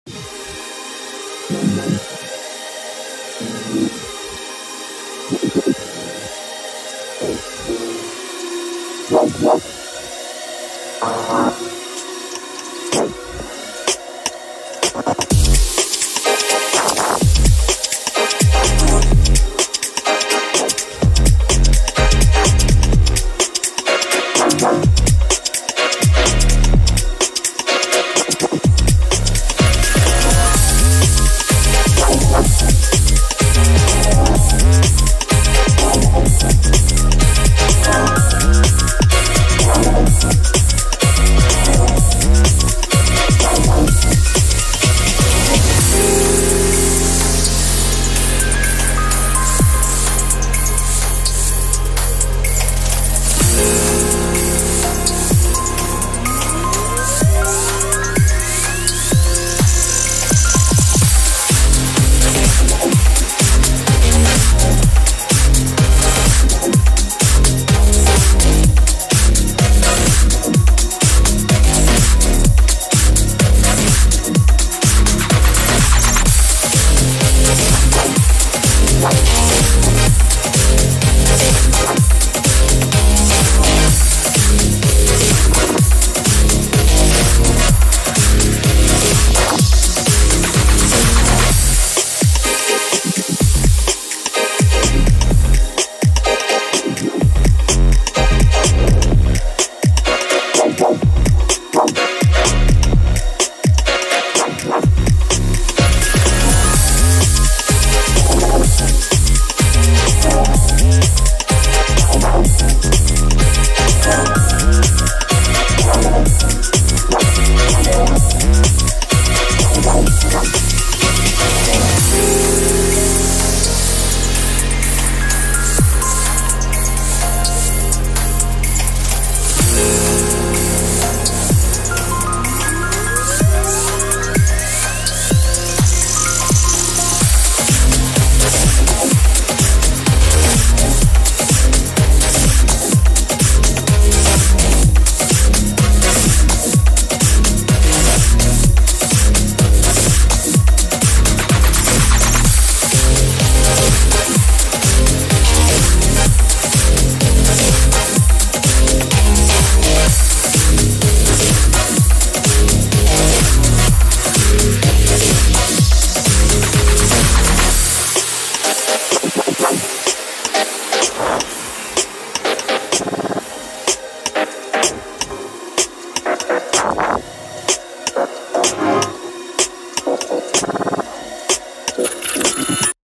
Can't Explain These Strange Feelings
I wasn't sure if this chord progression was a winner or not but I kept with it. Although I like the sound design and feelings I get overall, it may not be very popular. I'm going back and forth loving and hating this one so imma drop it like a hot plate and blame the cat.